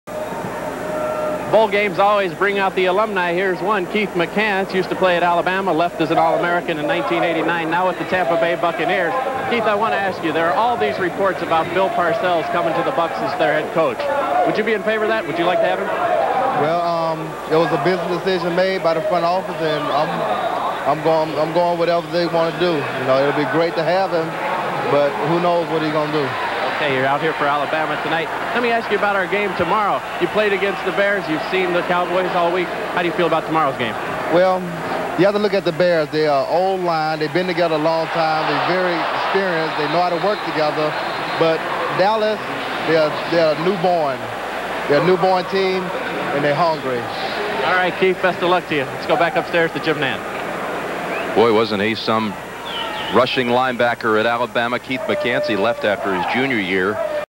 Keith McCants Interviewed From Alabama Sideline at 1991 Blockbuster Bowl
Interview by Jim Gray at the 1991 Blockbuster Bowl, a matchup between the Alabama Crimson Tide and the University of Colorado Buffaloes